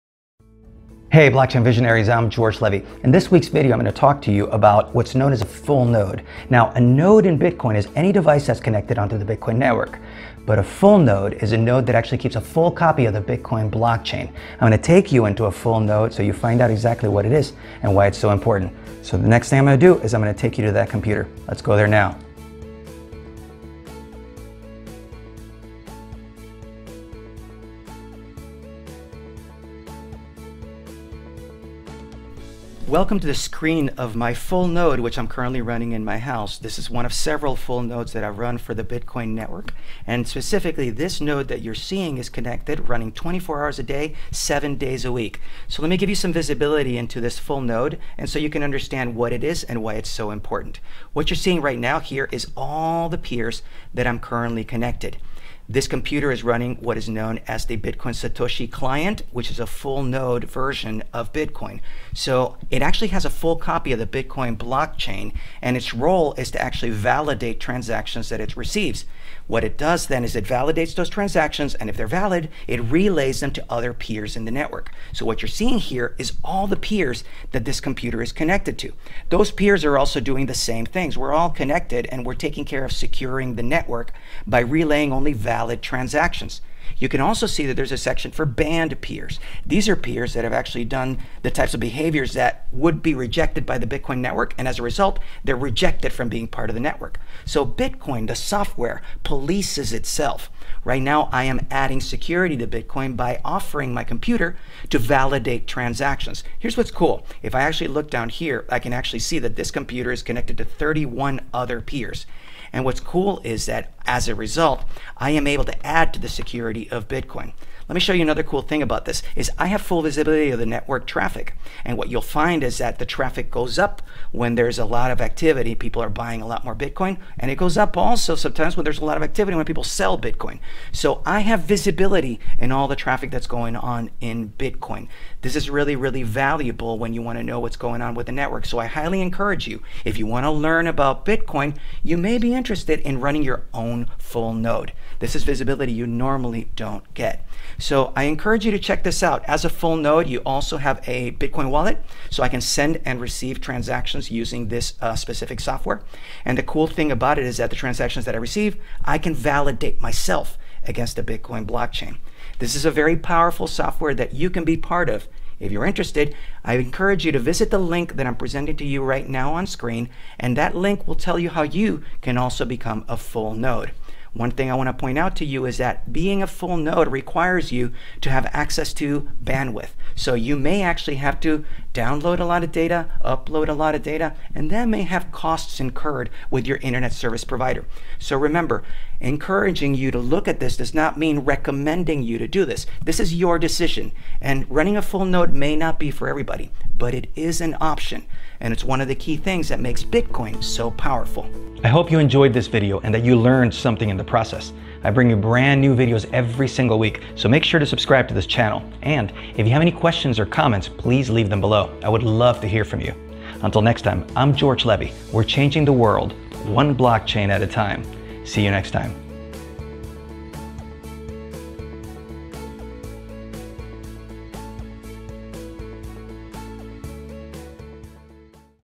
Running a Bitcoin Full Node: Things You Should Know. - George Levy
In this video, I take you onto one of my computers which is running a Bitcoin Full Node. In the process, I explain to you what a Bitcoin Full Node is, and how you can download and set up your own.  I also show you some of the functionality you get with a Full Node and  some of the key things you should know (and warnings) when you're running a Bitcoin Full Node.

Bitcoin: 3BFuf1axnpiPf7JM2CVqkfe8WbFhVkw2V5

Ethereum: 0xb32B1e64a75f54C6b0E86fF931DE9fcBa609C7d4

Litecoin: MTMbTXFN25A8RYQeWbypE2aaF1SEEsT9e6

Dogecoin: DPjEML99f45xRUyN6L1WyBAVXGPj5W2mqK